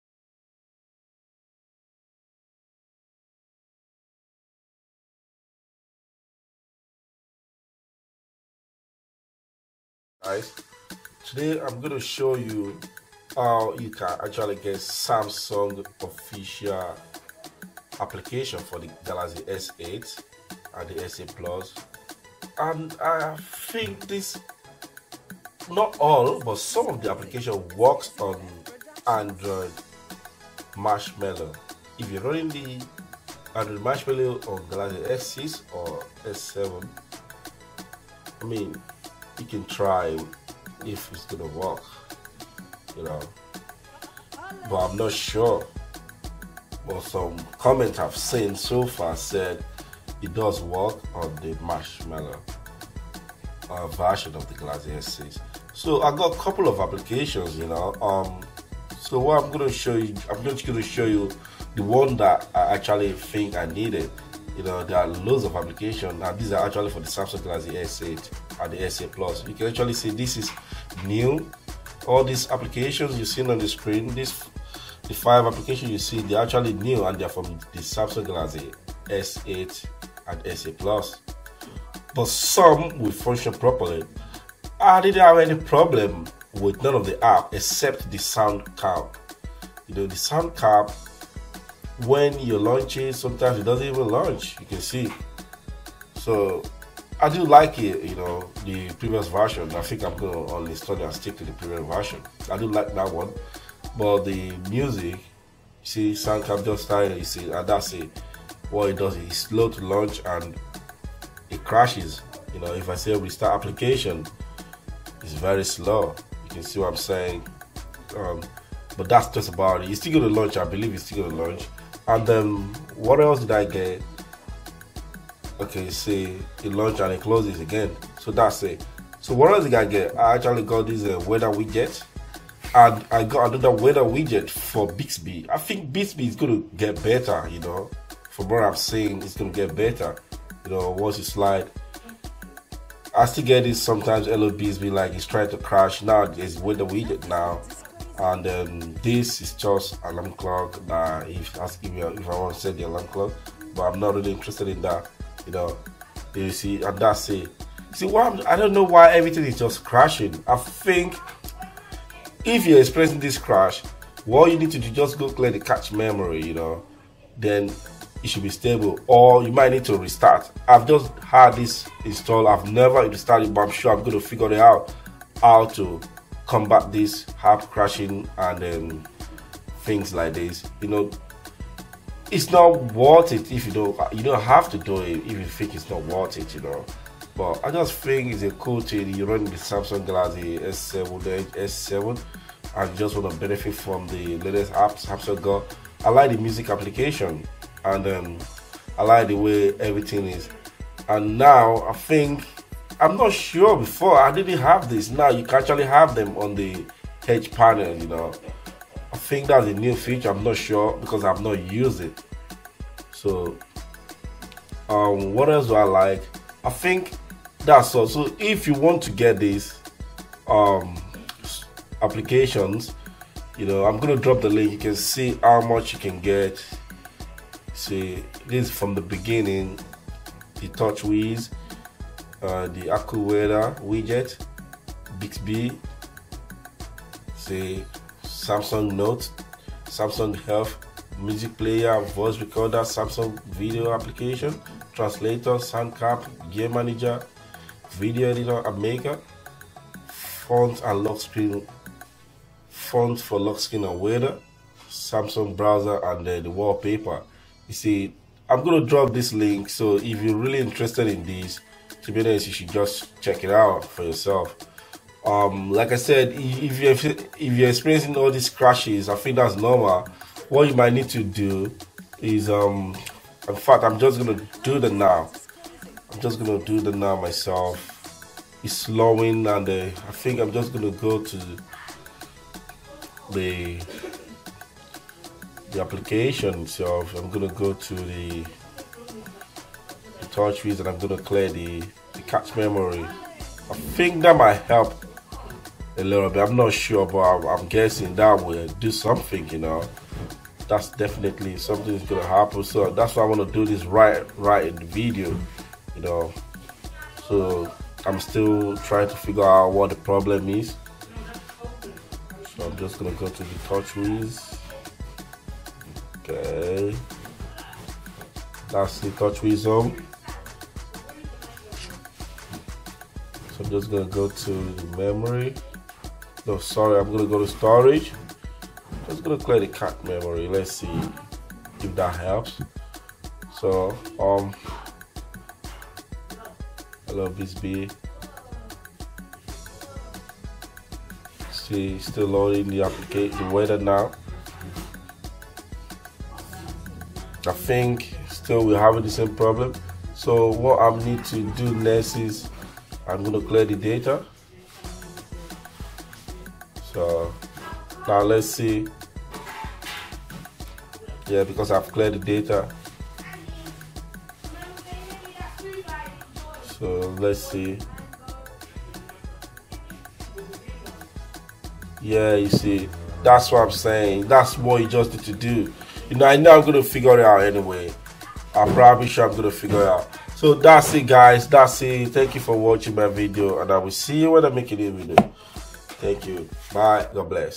Get Galaxy S8 Apps on S7 edge | No Root!!!
this is just an APK files, no need to root your phone, if you don't like it you can uninstall it.